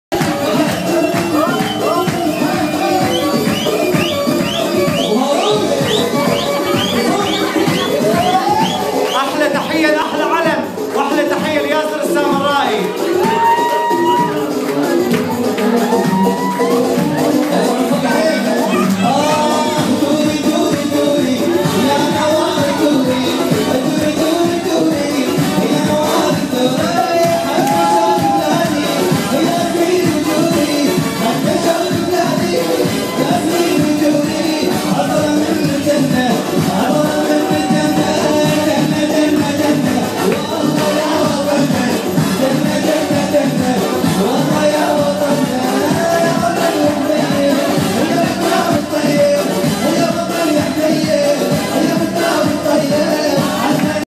حفلة عيد الحب من DJ Al Jazeera & DJ Samoha
حفلة عيد الحب من دي جي الجزيرة و دي جي سموحة في لندن
Valentine's party in London from DJ Jazeera and DJ Samoha
Arabic parties in London
Iraqi parties in London
Arabic wedding planners in London
Arabic female DJ in London